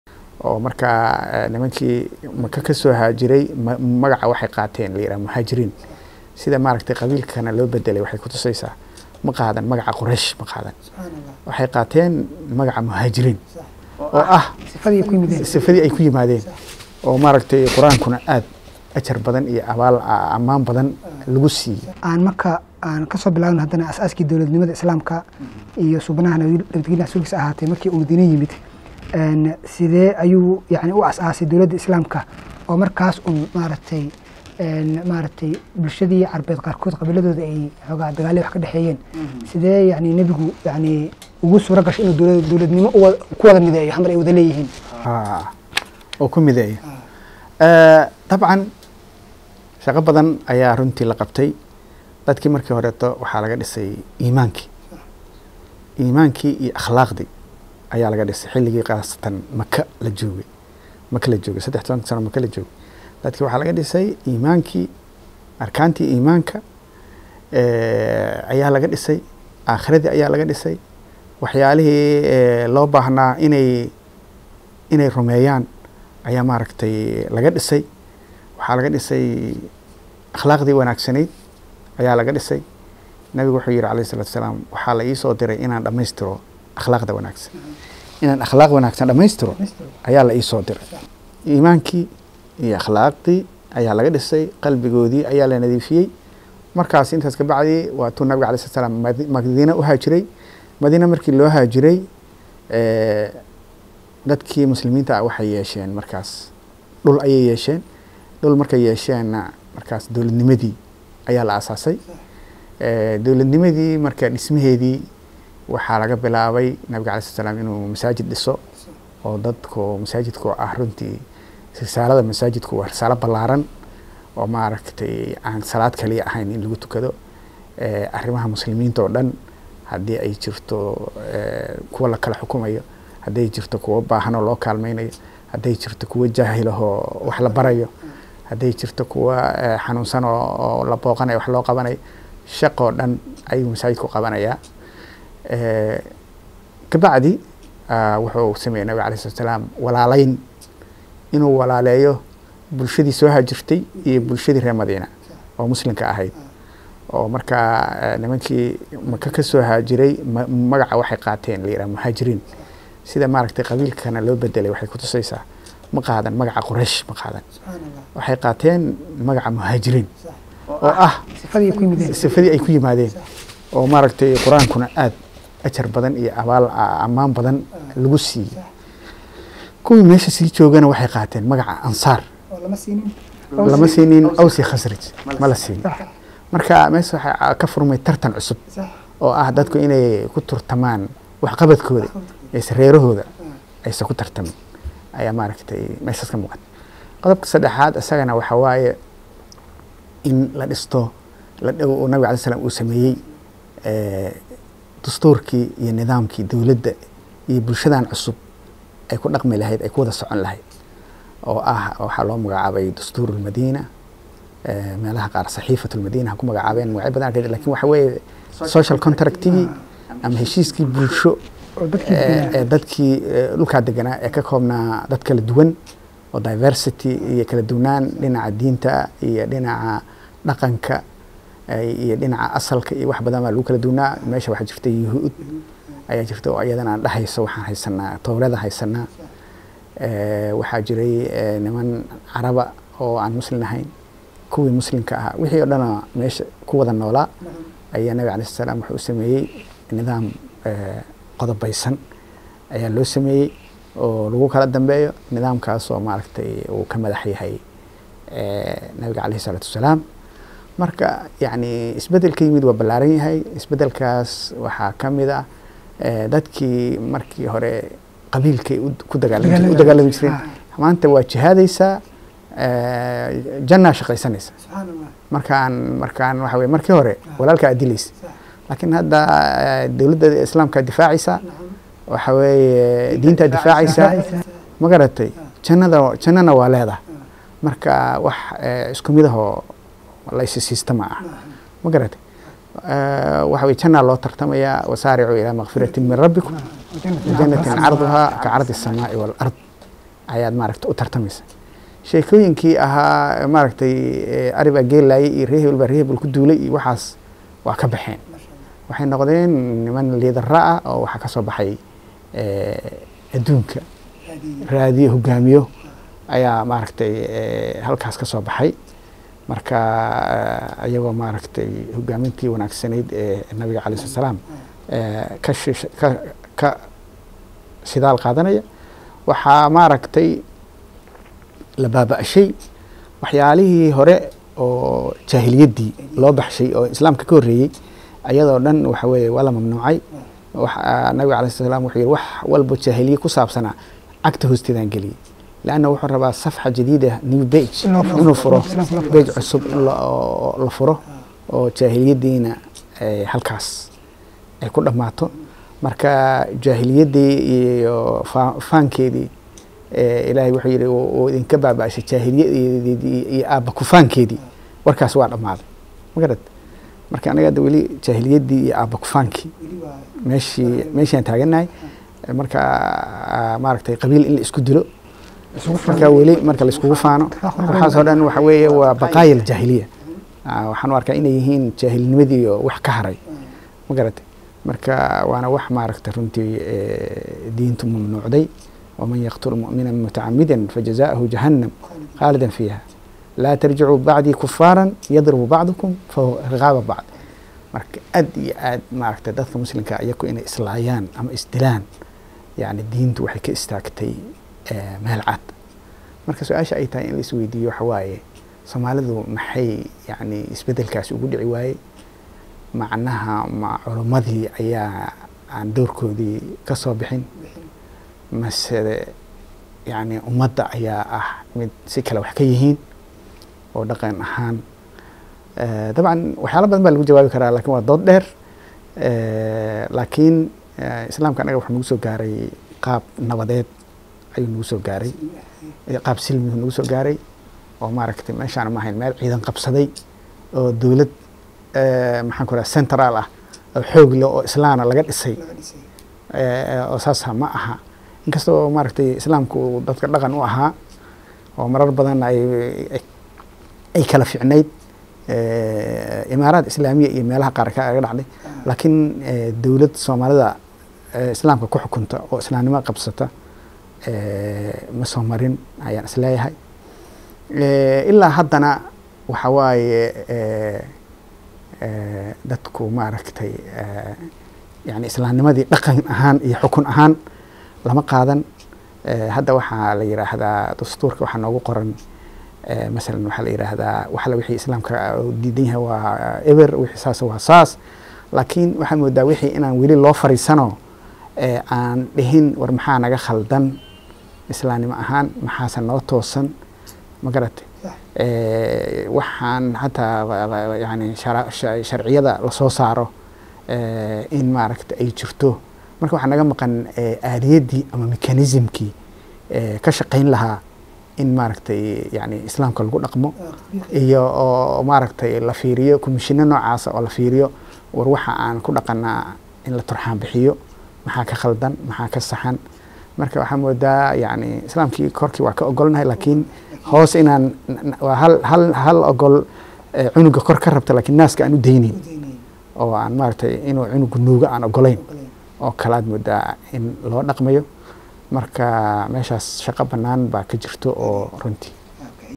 Magacyada Muhaajiriin iyo Ansaar iyo sida Nabigu u dhisay Umma Islaamiya oo Walaalnimadoodu xoogan..
Magacyada Muhaajiriin iyo Ansaar iyo sida Nabigu u dhisay Umma Islaamiya oo Walaalnimadoodu xoogan tahay!